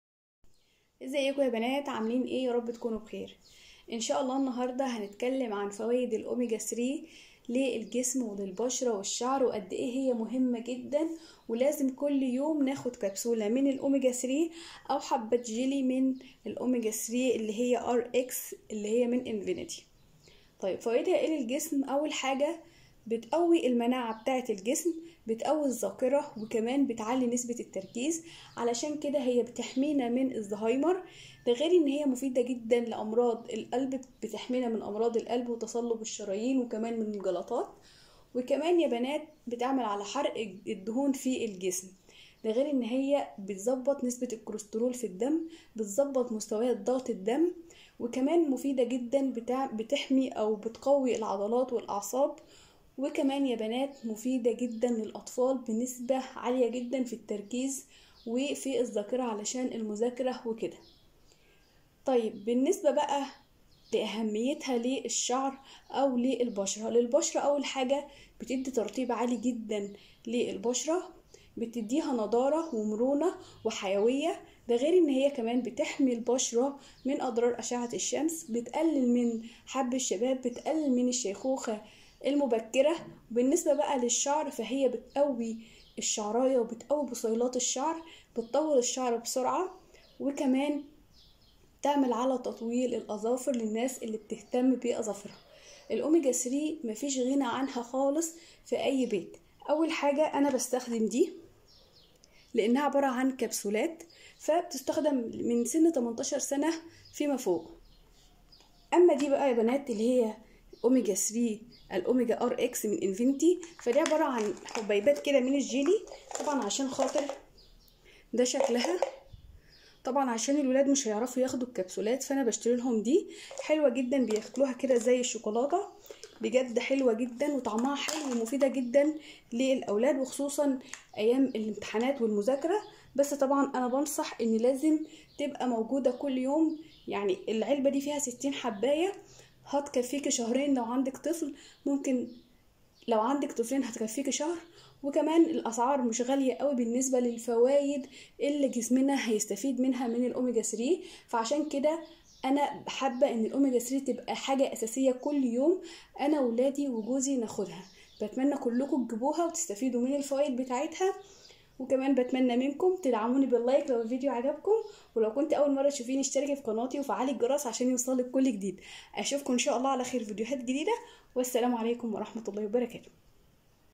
فوائد اوميجا 3 واميجاRXللاطفال والكبار وأهميتها للجسم والبشره والشعر هتفرق معاكي ومع اطفالك
فوائد اوميجا 3 واميجاRXللاطفال والكبار هتفرق معاكي ومع اطفالك لاغني عنها يومياً لتحسين الذاكره والتركيز والصحه قلبك وشعرك وبشرتك